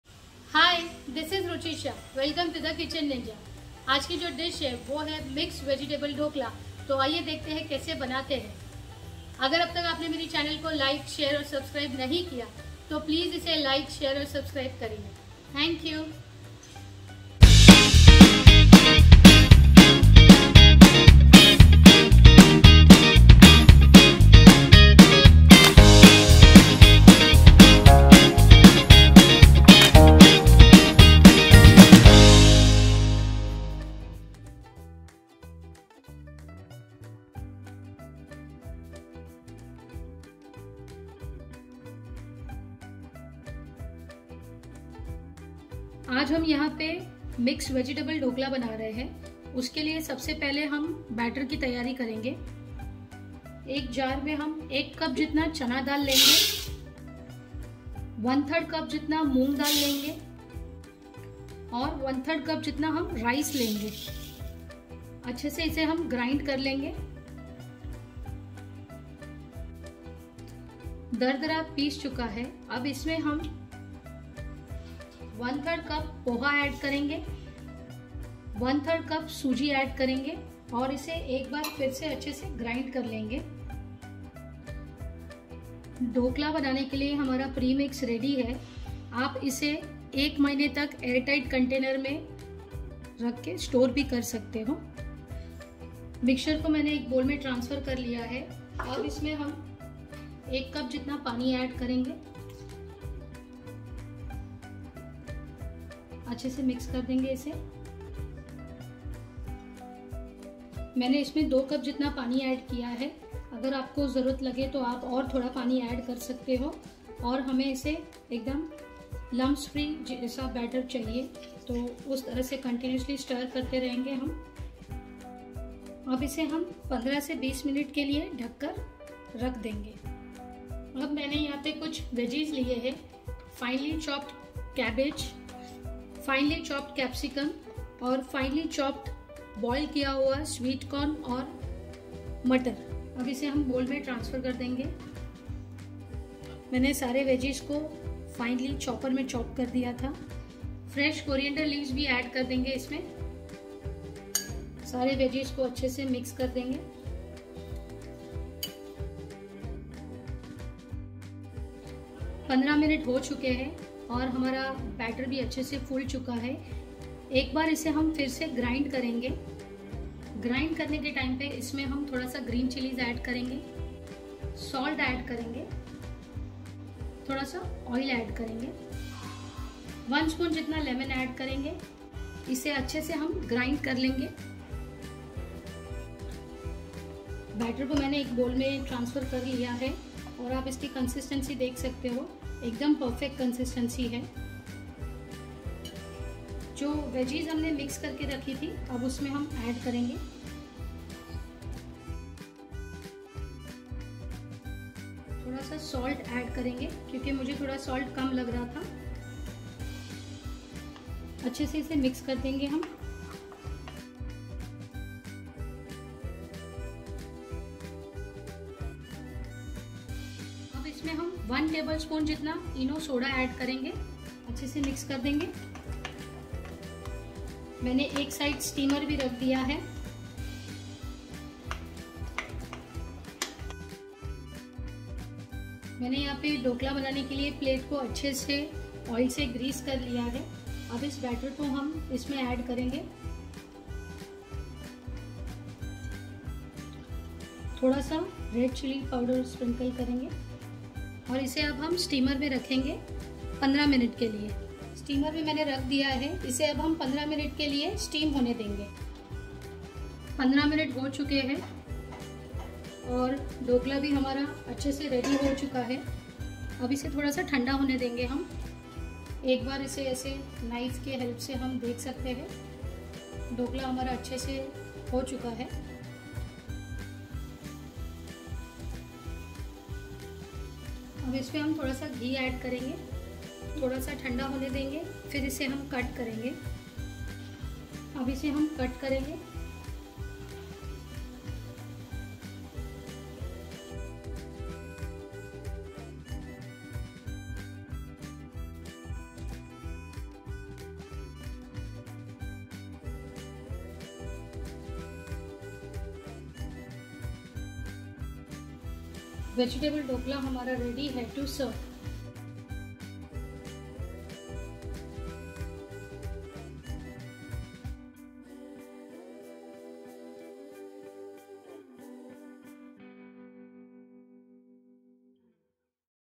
Mix vegetable Dhokla // homemade// vegetable// jain // recipe// yummy// healthy// By Ruchi shah
Here is the healthy version of Dhokla with lots of veggies in it...a perfect dish for tiffin.
U can store this premix for 1month.
Yes it's healthy vegetable Dhokla